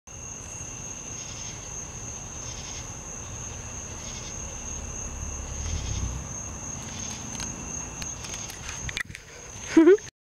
Just a Agrius cingulata (Pink-Spotted Hawkmoth)
Found this little creature on the wall outside and thought to take a little video of it.
Also called a Pink Sphinx Moth. It alit on my hand for a moment but mostly flew
into my face, hair and landed on my back a few times before I turned the light out.